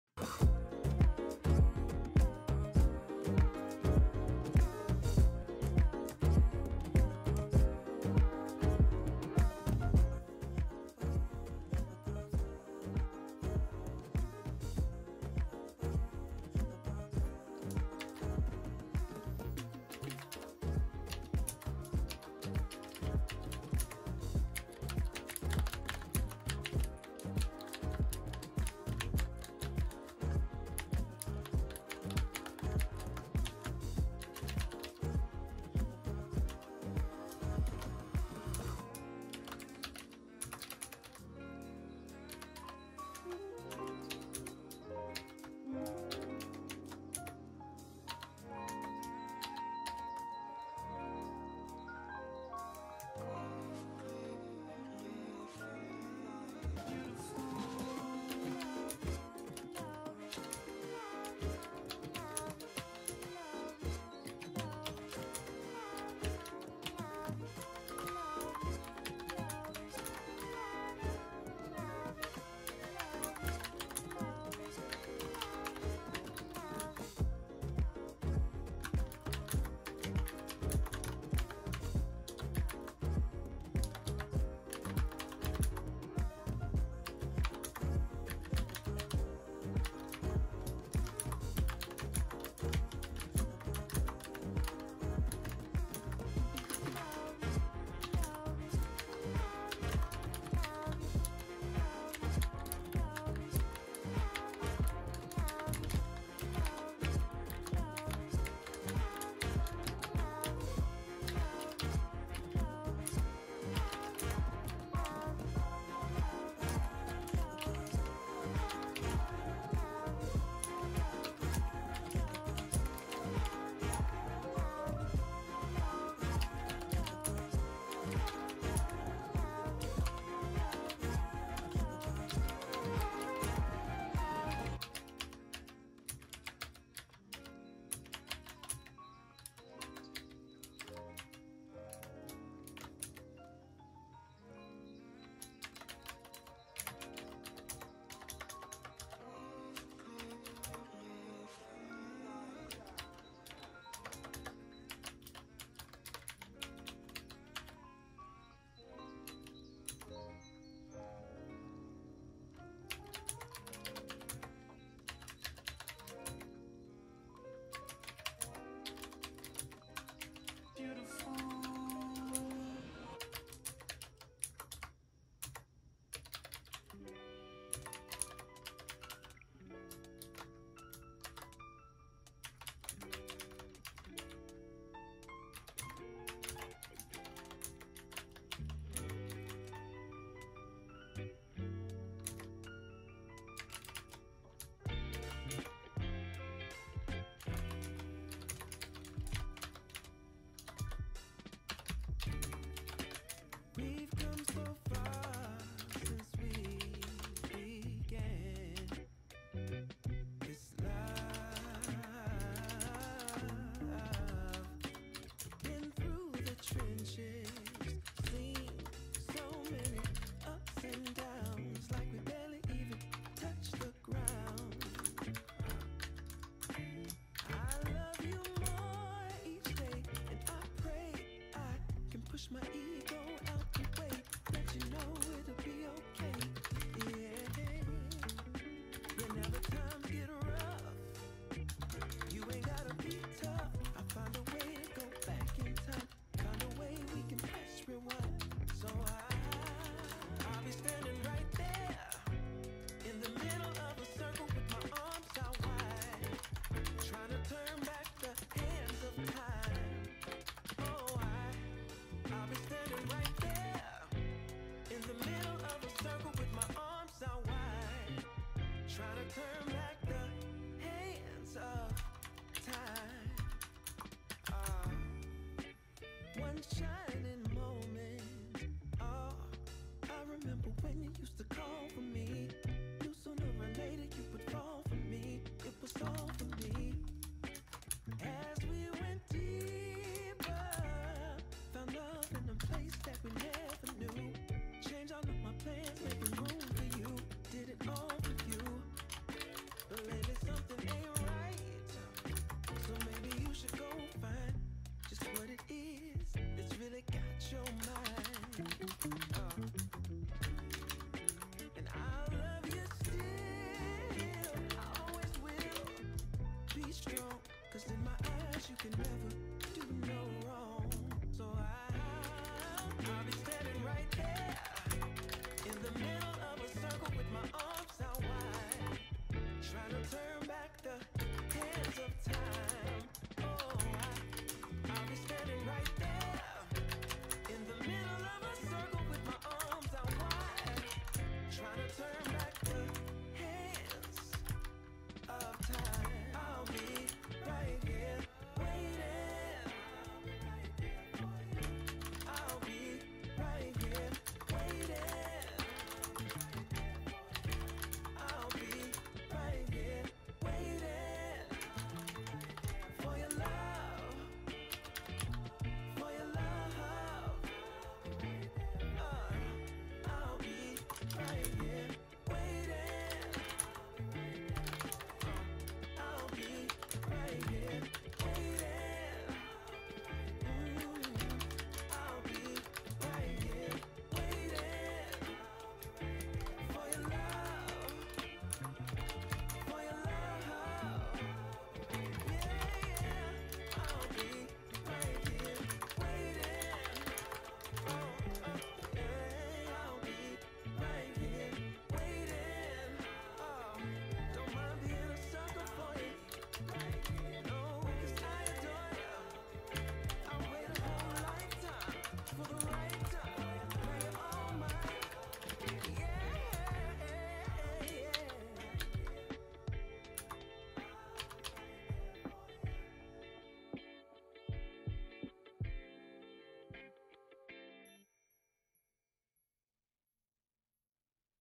【STUDY With Me】癒しのタイプライター#２⌇qwerkywriter ⌇retro typewriter keyboard ⌇
Hello, Muggle people!
【久しぶりにQwerkywriter keyboardで英語日記を作成しました📖🌿＃2】
どこか『レトロなイギリス🇬🇧』を感じられる美しいキーボード。

癒しの音楽とタイプライター音をお楽しみ下さい☺️💓